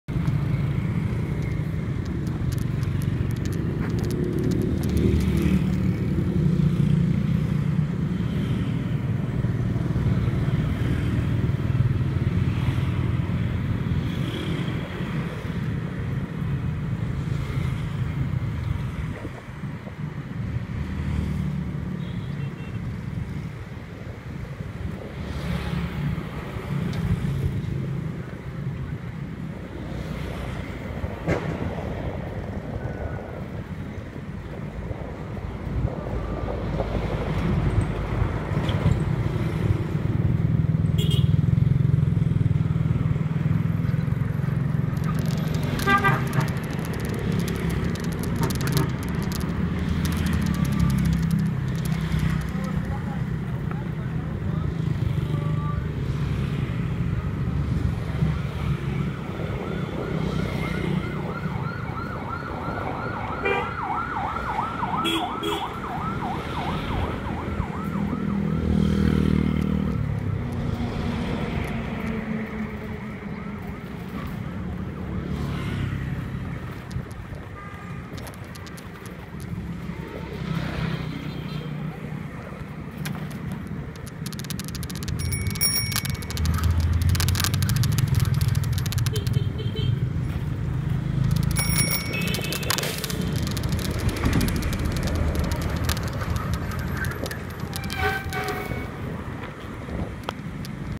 Ống Cống Và Phơi Nhang 2021.12.07 08:04
Quay trên Quốc Lộ 91 khi xuống Long Xuyên và thấy người đang phơi nhang luôn.